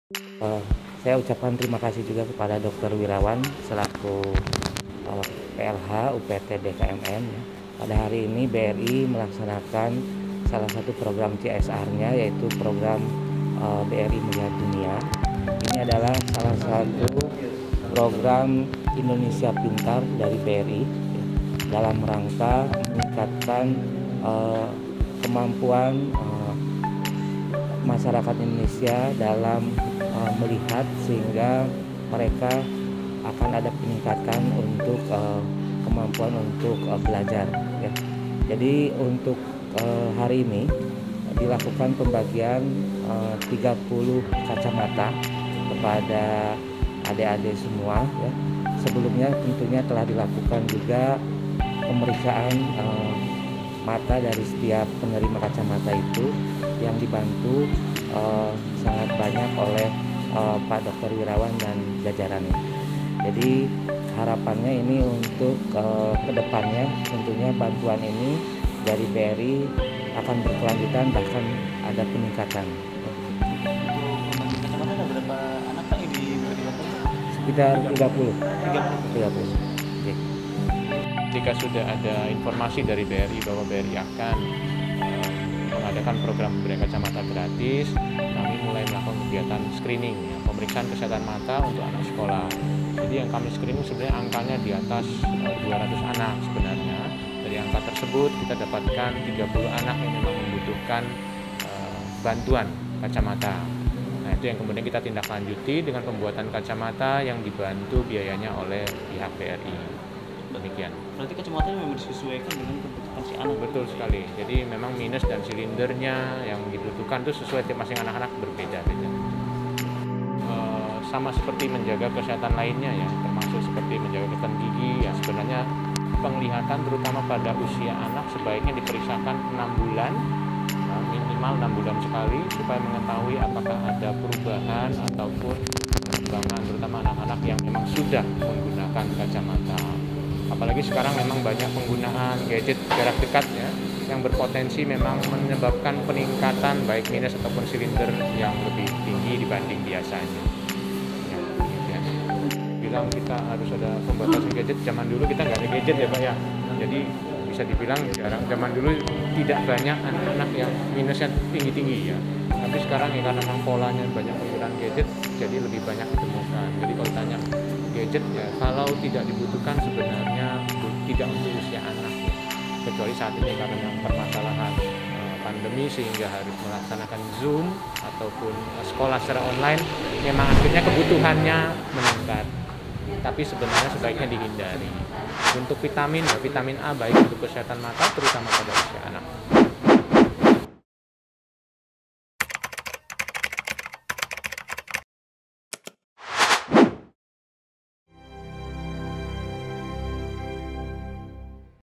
Program BRI Melihat Dunia, BRI Cabang Pontianak Beri Kacamata ke 30 Pelajar di Pontianak
Bank Rakyat Indonesia (BRI) memberikan kaca mata gratis bagi 30 anak - anak di Kota Pontianak yang mengalami gangguan penglihatan.

Dalam program BRI melihat dunia, BRI Cabang Pontianak Barito turut menggandeng Balai Kesehatan Mata Masyarakat (BKMM) Kota Pontianak.

Satu per satu anak - anak yang berasal dari berbagai sekolah dasar di Kota Pontianak di beri kacamata yang sesuai dengan kebutuhan mereka.

Kendati menggunakan masker saat menerima kacama tersebut karena penerapan protokol kesehatan Covid 19 yang ketat, kebahagiaan elas terpancar dari wajah anak - anak tersebut.